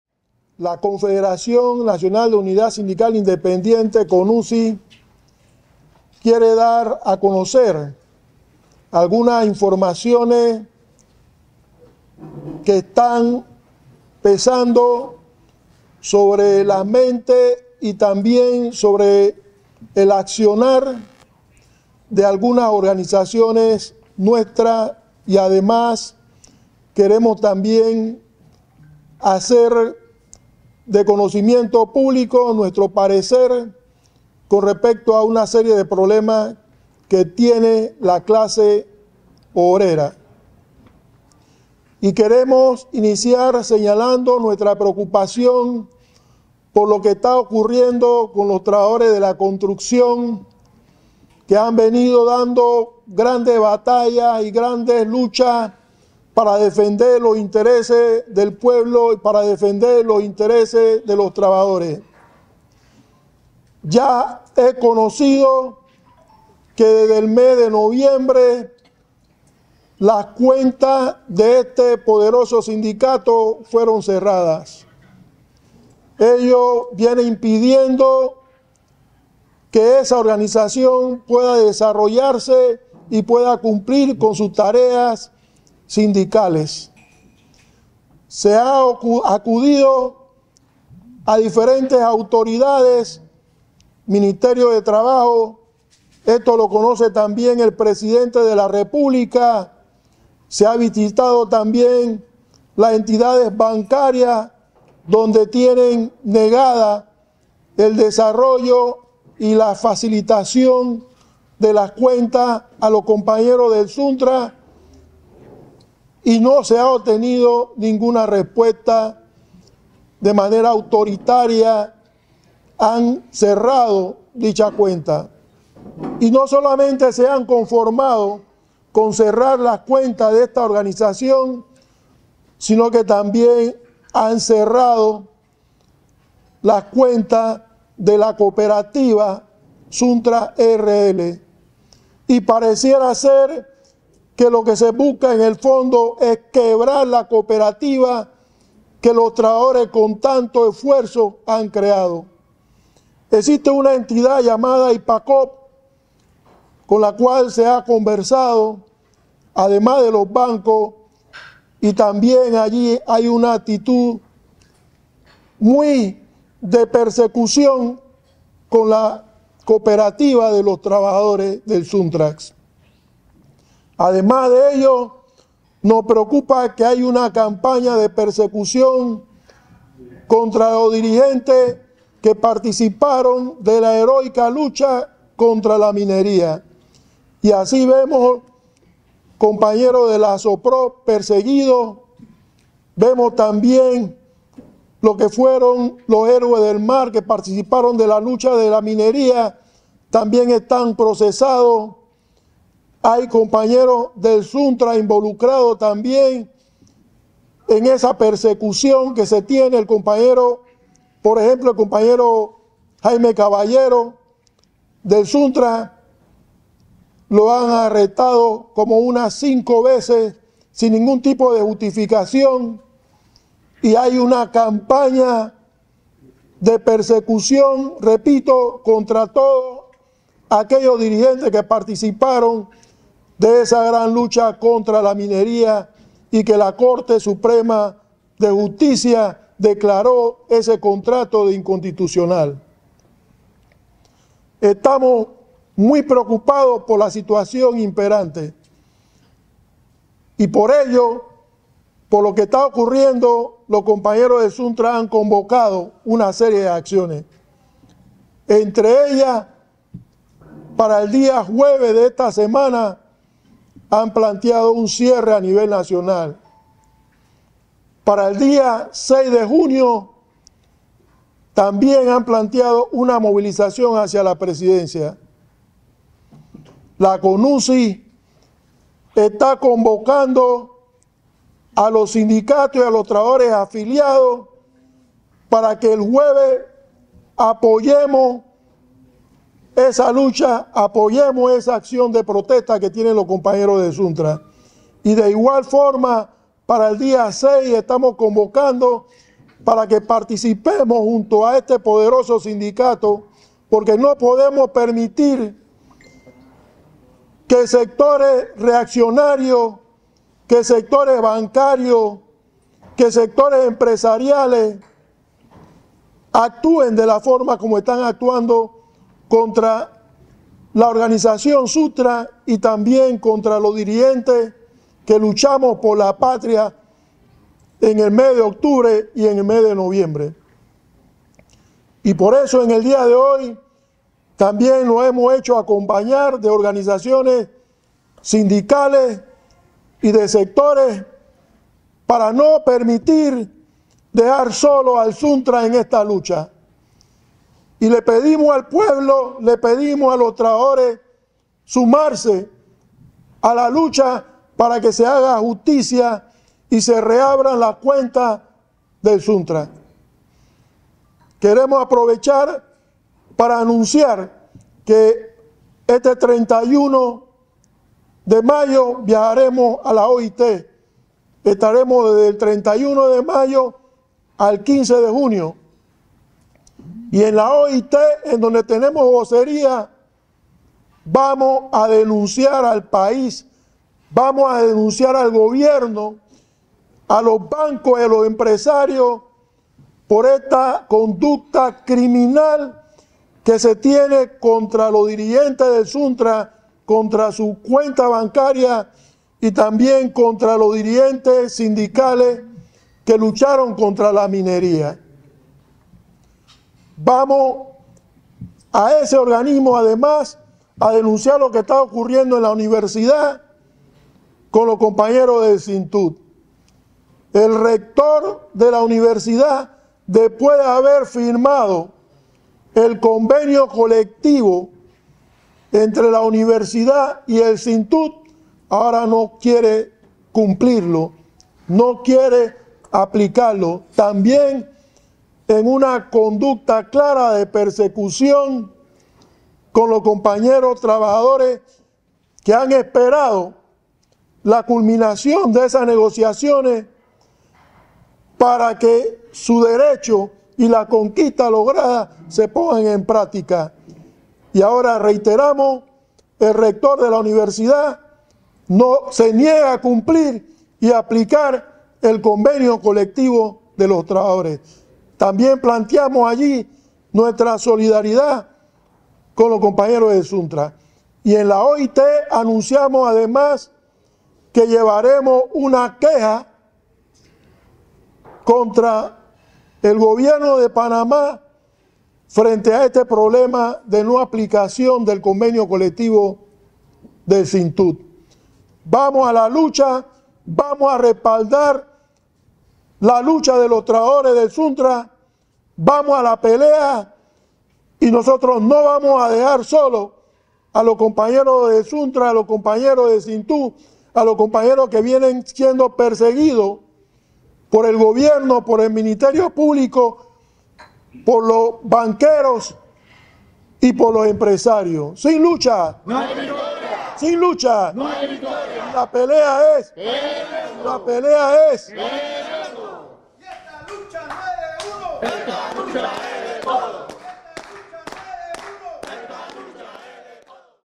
Conferencia con la Prensa CONUSI 2852024
Conferencia de prensa CONUSI en solidaridad con SUNTRACS por cierre de sus cuentas, contra persecución a lancheros, educadores, ambientalistas, sindicalistas, estudiantes, originarios y campesinos por su participación en gesta contra contrato minero.